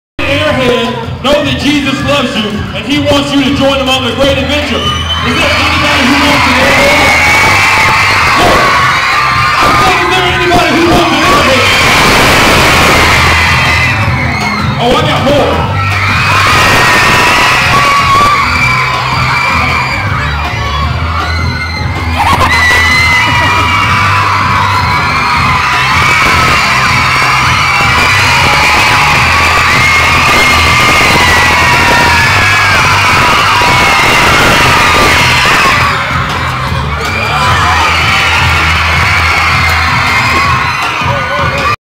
Hey Ya getting the kids worked up at Kids Quest.
Hey Ya throwing candy out to the kids.